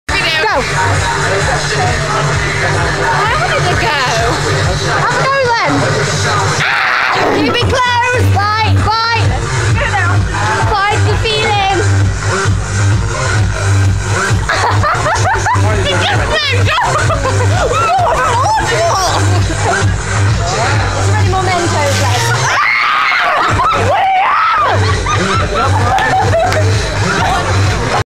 Mentos & Coke challenge
So maybe we used coke not diet coke but drink enough and apparently the same thing happens. It's beautiful.